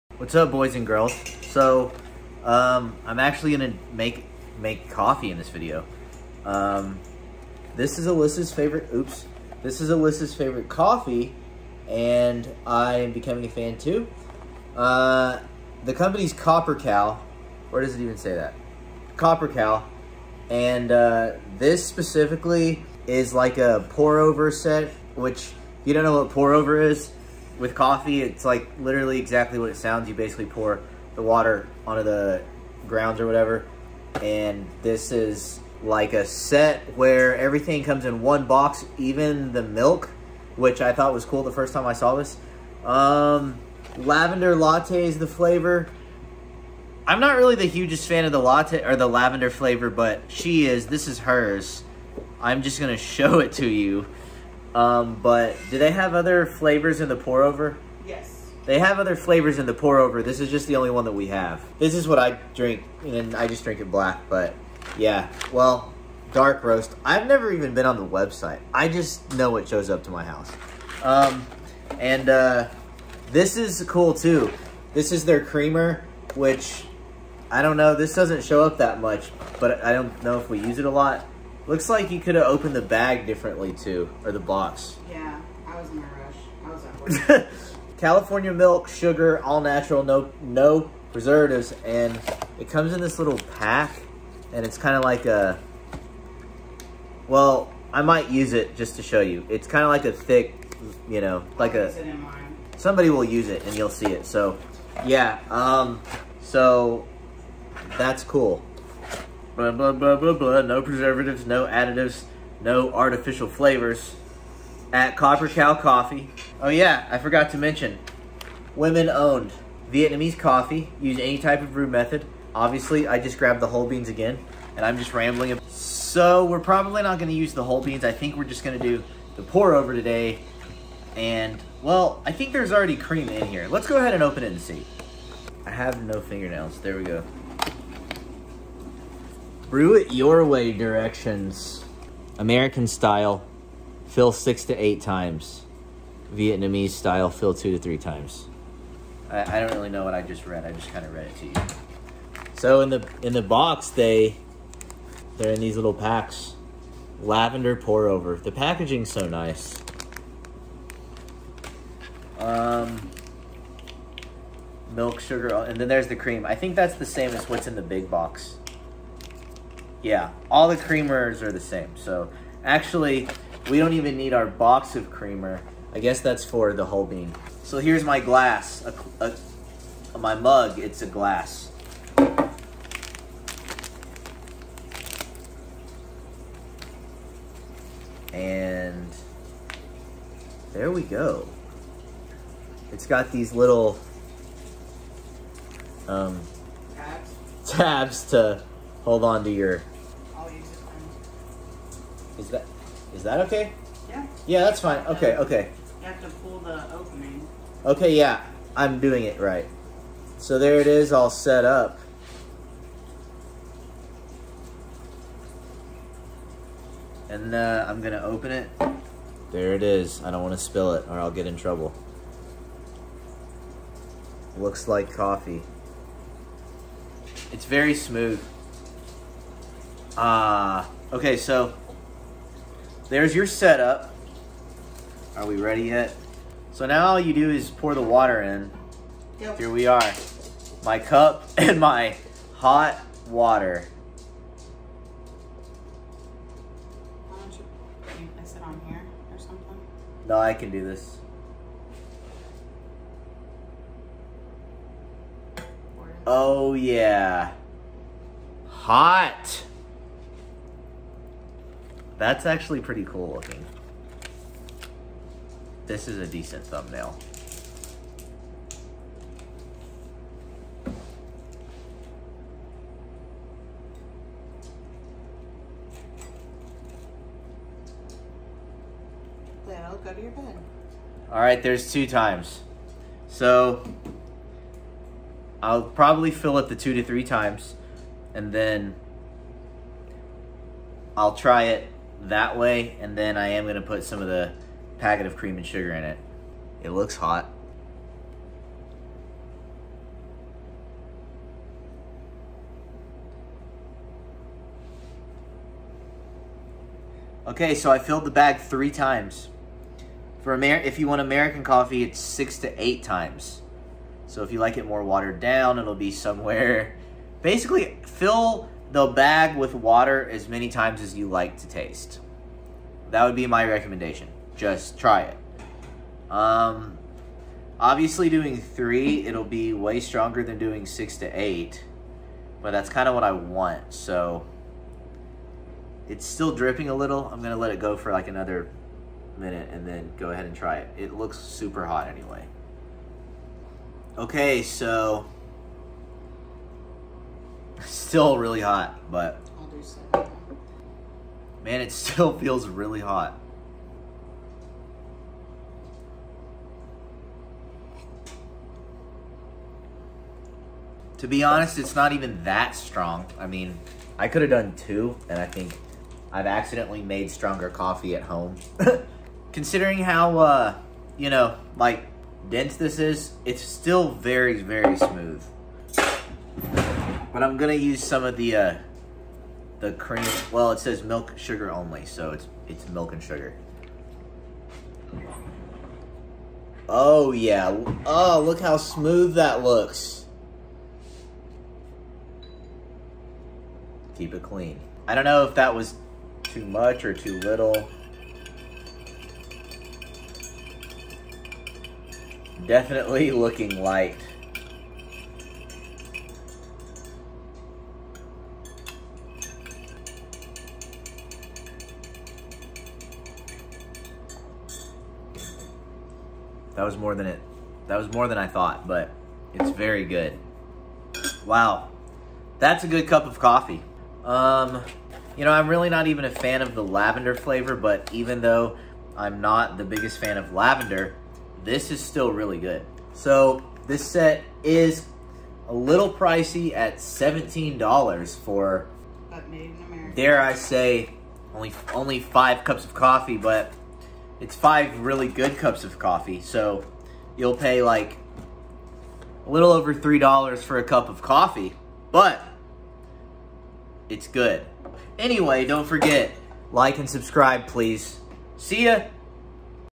Copper Cow Coffee LAVENDER LATTE POUR OVER SET | How-To, Unboxing & Review
I unbox and make Copper Cow Coffee's Lavender Latte Pour Over Set. An awesome company making awesome coffee.